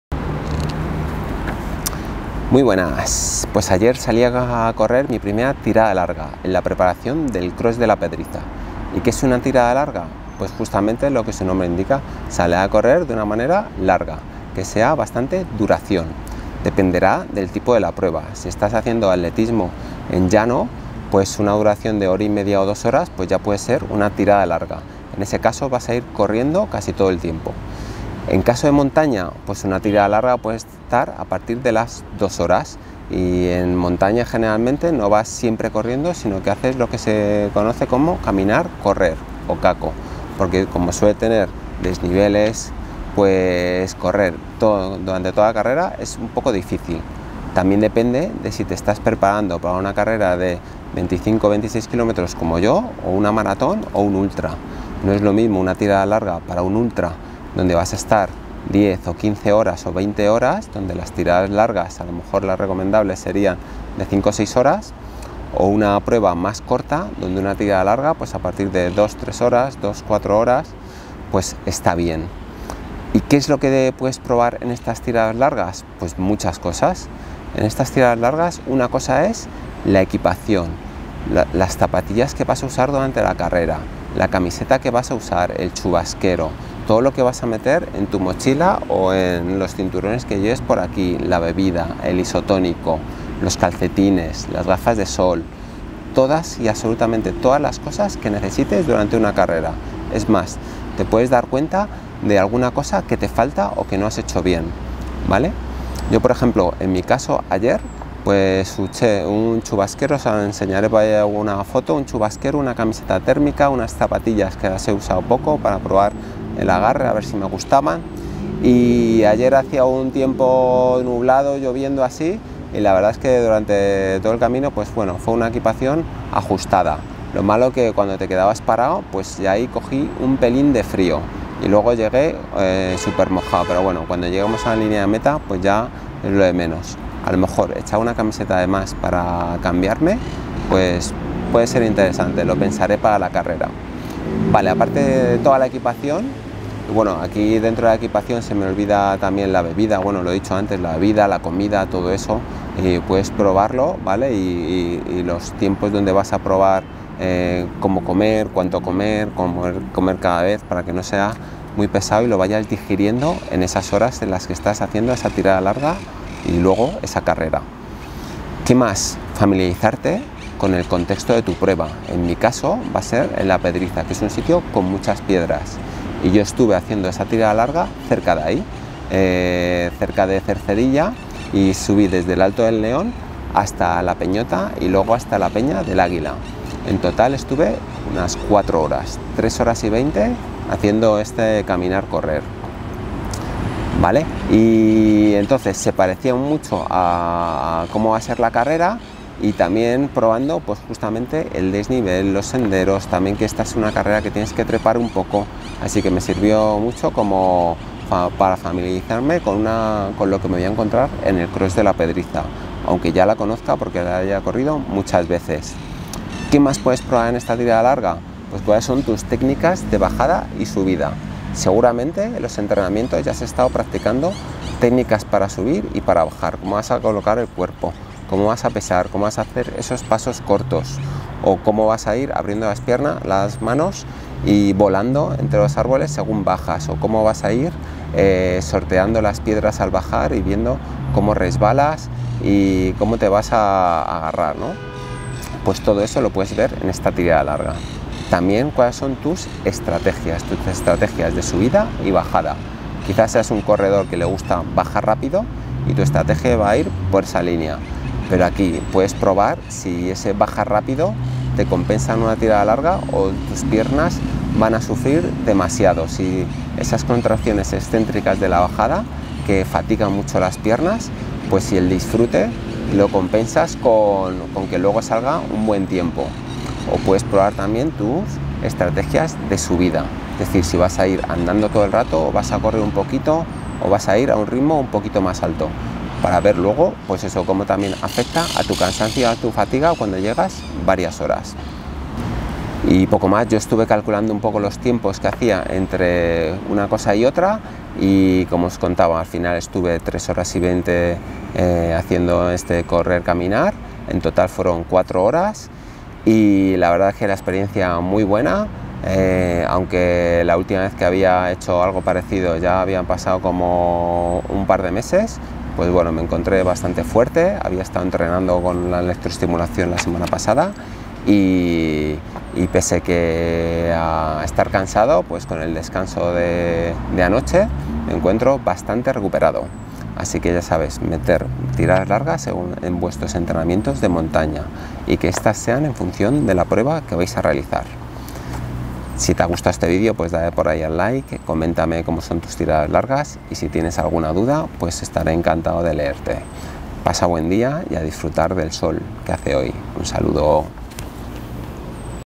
👉TIRADAS LARGAS en entrenamiento de Carrera montaña - 🏃‍♂️ CROSS PEDRIZA 2021 VLOG
Entrenar salidas largas para carreras de montaña.